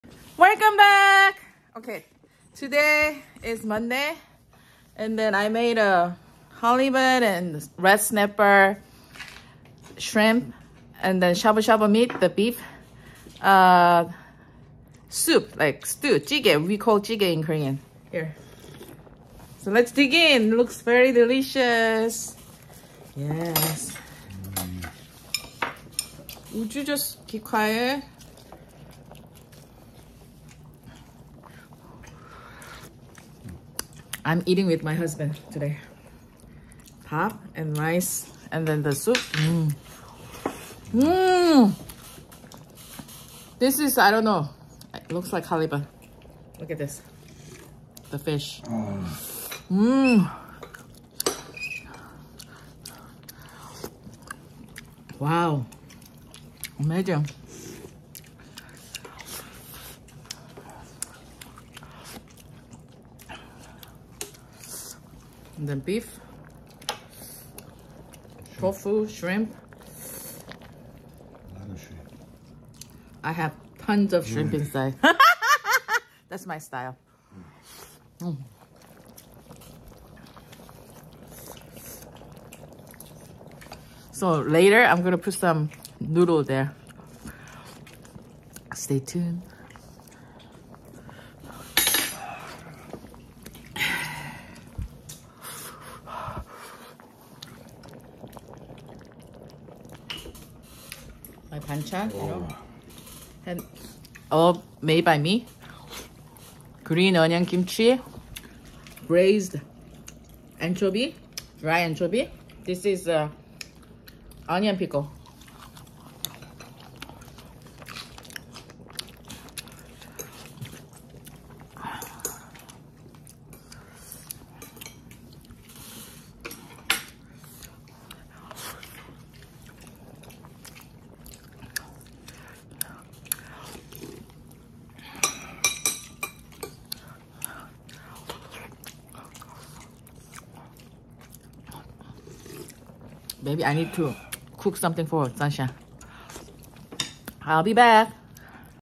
Spicy Halibut, Shrimp, &Beef Soup Eating Mukbang Show~🍚😎🙋‍♀️
Hi everyone,
I cooked hot and spicy halibut, shrimp, and beef soup since I was craving for that. I just love spicy and hot seafood soup so much~
This kind of seafood soup is very light and fills you up good. Also it can be the diet food due to have almost zero cholesterol. High in good fish and beef protein makes your body more youthful~
For hot and spicy seafood soup, dissolve red pepper paste in the generous amount of water in the pot. Put the radish and bring that to boil.
Add choice of your seafood like fish, shrimp, clam, calamari, etc..., onion, garlic, tofu, green onion, crown daisy, water parsley, and perilla leaves(optional).
Season with salt, white pepper, and red pepper.
Serve when it is hot and you may add any type of noodles to the soup(optional).
Let's be well and God bless you all~

*I am uploading this video on 6/02/22 and my husband and I let Sunshine rest in peace. She was really sick and we couldn't hold on to her.
Since I mentioned I was feeding Sunshine the shrimp in the video and I just let you guys know, she no longer exists.